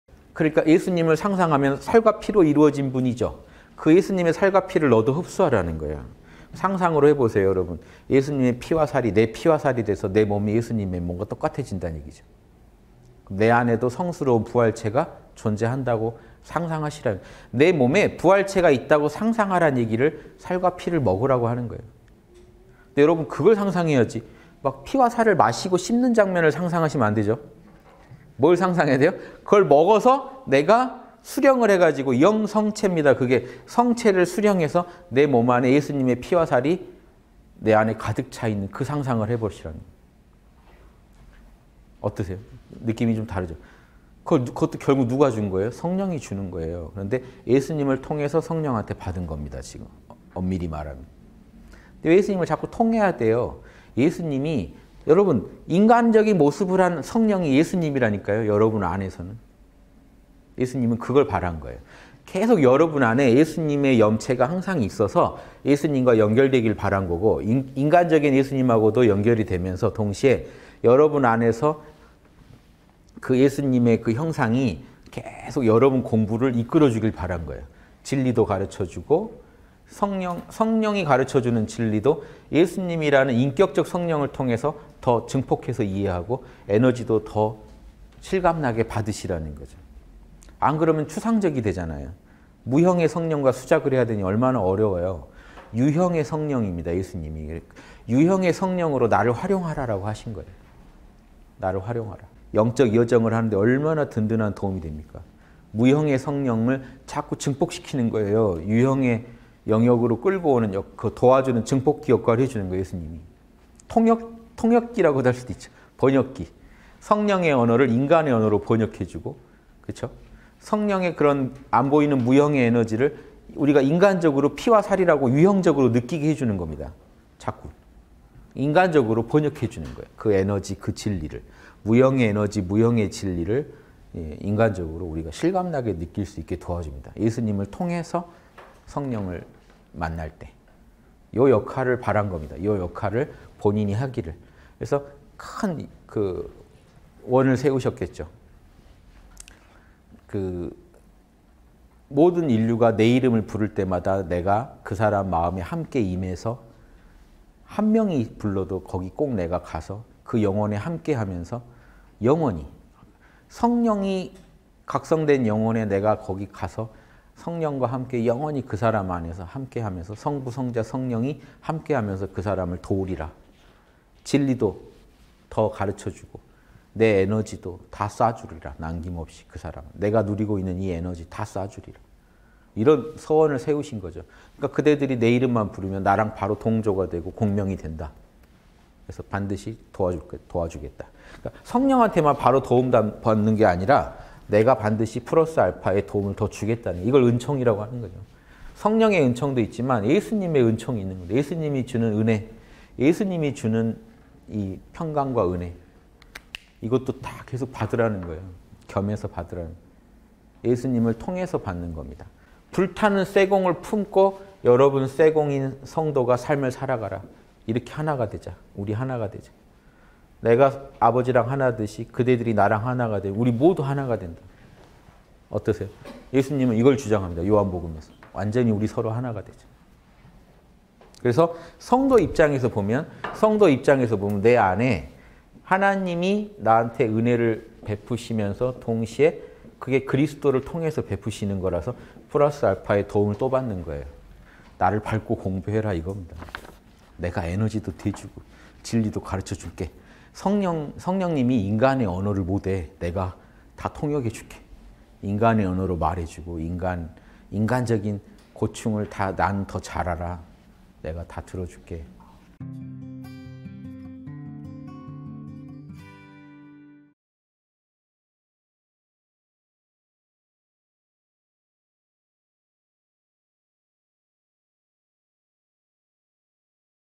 [3분 양심] 예수님의 원, 내 살과 피를 먹어라!
- (190413 성령과 구원) 예수님의 원, 내 살과 피를 먹어라!
그러니까 예수님을 상상하면
‘살과 피로 이루어진 분’이죠.
“그 예수님의 살과 피를 너도 흡수하라”는
거예요. 상상으로 해 보세요, 여러분.
예수님의 피와 살이 내 피와 살이 돼서
내 몸이 예수님의 몸과 똑같아진다는 얘기죠.
내 안에도 성스러운 부활체가 존재한다고
상상하시라는 거예요.
내 몸에 부활체가 있다고 상상하라는 얘기를
“살과 피를 먹으라”고 하는 거예요.
여러분, 그걸 상상해야지, 막 피와 살을 마시고
씹는 장면을 상상하시면 안 되죠.

뭘 상상해야 돼요?
그걸 먹어서, 내가 수령을 해 가지고
영성체(領聖體)입니다, 그게. 성체를
수령해서 내 몸 안에 예수님의 피와 살이
내 안에 가득 차 있는 그 상상을
해 보시라는 거예요. 어떠세요?
느낌이 좀 다르죠?
그것도 결국 누가 준 거예요?
성령이 주는 거예요.
그런데 예수님을 통해서 성령한테
받은 겁니다, 지금, 엄밀히 말하면.
그러니까 예수님을 자꾸 통해야 돼요.

여러분, ‘인간적인 모습을 한 성령’이
예수님이라니까요, 여러분 안에서는.
예수님은 그걸 바란 거예요.
계속 여러분 안에 예수님의 염체(念體)가
항상 있어서 예수님과 연결되기를 바란 거고,
인간적인 예수님하고도 연결이 되면서 동시에,
여러분 안에서 그 예수님의 그 형상이
계속 여러분의 공부를 이끌어 주길 바란 거예요.

진리도 가르쳐 주고,
성령이 가르쳐 주는 진리도
예수님이라는 인격적 성령을 통해서
더 증폭해서 이해하고,
에너지도 더 실감 나게 받으시라는 거죠.
안 그러면 추상적으로 되잖아요.
무형의 성령과 수작을 해야 되니 얼마나 어려워요?
유형의 성령입니다, 예수님이.
유형의 성령으로 “나를 활용하라!”
라고 하신 거예요. “나를 활용하라!”
영적 여정을 가는데 얼마나 든든한 도움이 됩니까?
무형의 성령을 자꾸 증폭시키는 거예요.

유형의 영역으로 끌고 오는, 도와주는
증폭기 역할을 해주는 거예요, 예수님이.
통역기라고도 할 수도 있죠. 번역기.
성령의 언어를 인간의 언어로 번역해 주고.
그렇죠? 성령의 그런 안 보이는 무형의
에너지를 우리가 인간적으로 피와 살이라고
유형적으로 느끼게 해주는 겁니다.

자꾸 인간적으로 번역해 주는 거예요,
그 에너지, 그 진리를.
무형의 에너지, 무형의 진리를 인간적으로
우리가 실감 나게 느낄 수 있게 도와줍니다.
예수님을 통해서 성령을 만날 때 이 역할을
바란 겁니다. 이 역할을 본인이 하기를.
그래서 큰 원(願)을 세우셨겠죠?

“모든 인류가 내 이름을 부를 때마다
내가 그 사람 마음에 함께 임해서
한 명이 불러도 거기 꼭 내가 가서
그 영혼에 함께하면서 영원히,
성령이 각성된 영혼에 내가 거기 가서
성령과 함께 영원히 그 사람 안에서 함께 하면서
성부·성자·성령이 함께 하면서
그 사람을 도우리라! 진리도 더
가르쳐 주고 내 에너지도 다 쏴 주리라.
남김없이 그 사람에게 내가 누리고 있는
이 에너지 다 쏴주리라!”

이런 서원(誓願)을 세우신 거죠.
“그러니까 그대들이 내 이름만 부르면
나랑 바로 동조가 되고 공명이 된다.
그래서 반드시 도와주겠다.” 그러니까
성령한테만 바로 도움 받는 게 아니라,
“내가 반드시 플러스알파(＋α)의
도움을 더 주겠다”는 것,
이걸 ‘은총’이라고 하는 거죠.
성령의 은총도 있지만 예수님의 은총이 있는 거죠.

예수님이 주는 은혜, 예수님이 주는
이 평강(平康)과 은혜.
이것도 다 계속 받으라는 거예요.
겸해서 받으라는 거예요.
예수님을 통해서 받는 겁니다.
“불타는 쇠공을 품고, 여러분,
쇠공인 성도가 삶을 살아가라.
이렇게 하나가 되자! 우리 하나가 되자!
내가 아버지랑 하나이듯이 그대들이
나랑 하나가 되면 우리 모두 하나가 된다!”
어떠세요? 예수님은 이걸 주장합니다,
『요한복음』에서.
“완전히 우리 서로 하나가 되자!”

그래서 성도의 입장에서 보면 내 안에서
하나님이 나한테 은혜를 베푸시면서 동시에
그게 그리스도를 통해서 베푸시는 거라서
플러스알파의 도움을 또 받는 거예요.
“나를 밟고 공부해라.” 이겁니다.
“내가 에너지도 대 주고 진리도 가르쳐 줄게!
성령님이 인간의 언어를 못해. 내가
다 통역해 줄게, 인간의 언어로 말해 주고.
인간적인 고충을 나는 더 잘 알아.
내가 다 들어 줄게!”